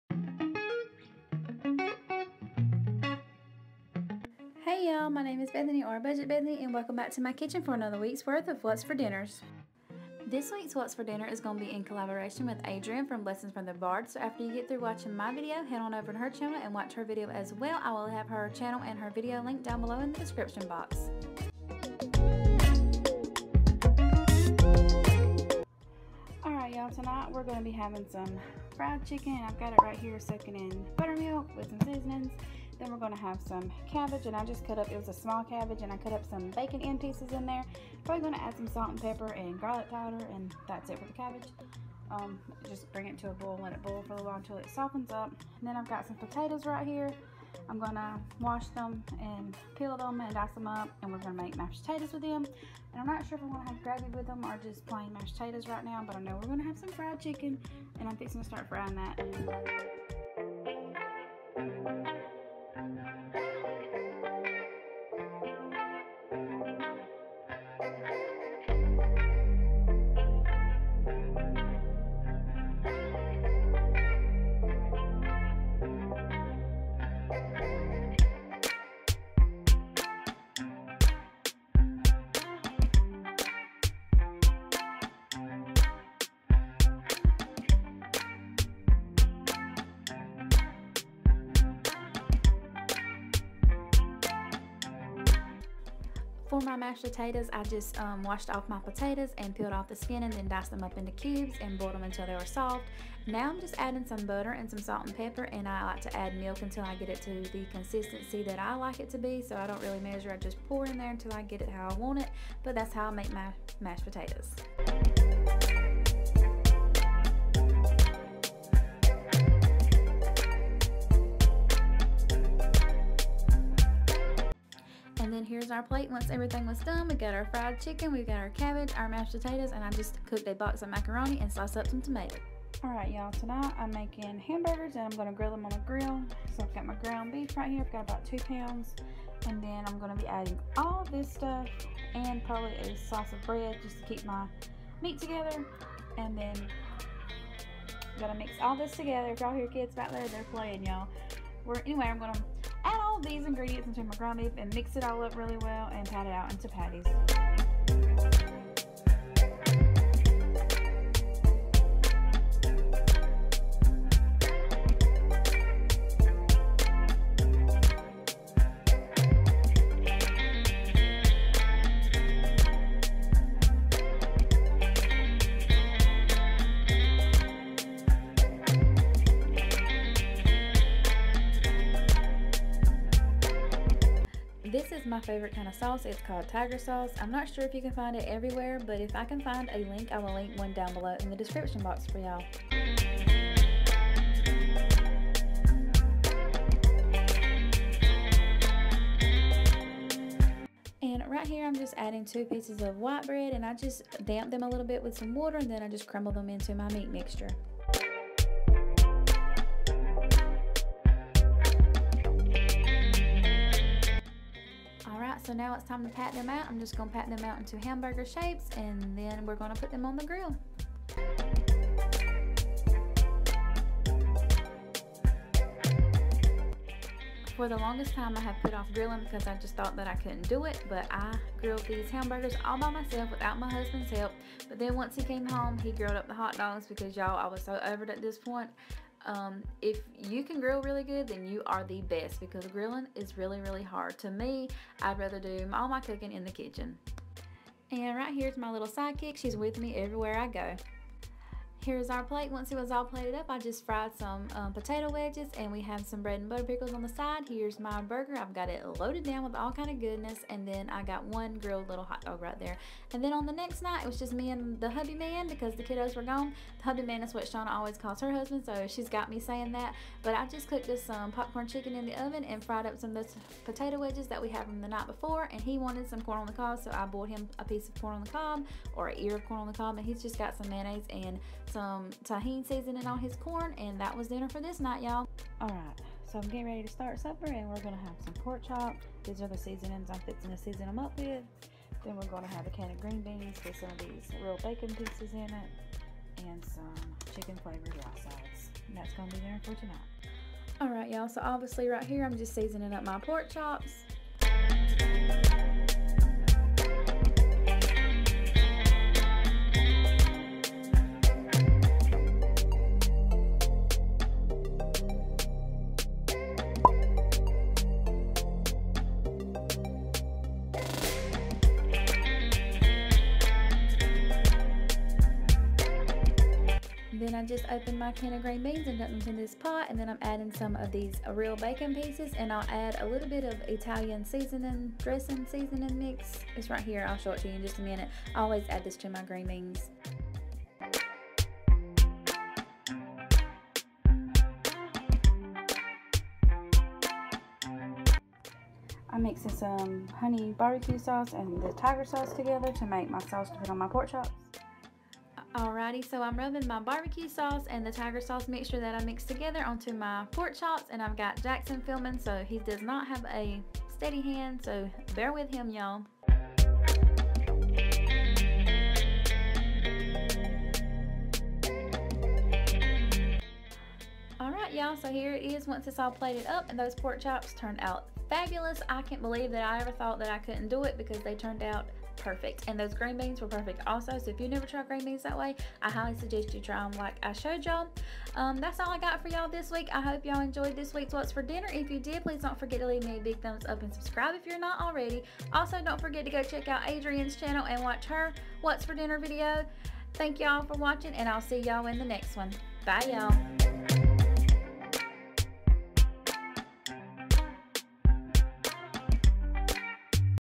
WHAT’S FOR DINNER | GIRLS CAN GRILL TOO
Welcome back to another What’s for dinner. Today’s video is in collab with Adrian from Blessings For The Bards. I will be sharing 4 dinner ideas with y’all this week. I hope you all enjoy.
Bethanie

If you’re interested in trying Tiger Sauce you can find it on Walmart.com

Facebook: Budget Bethanie’s Bites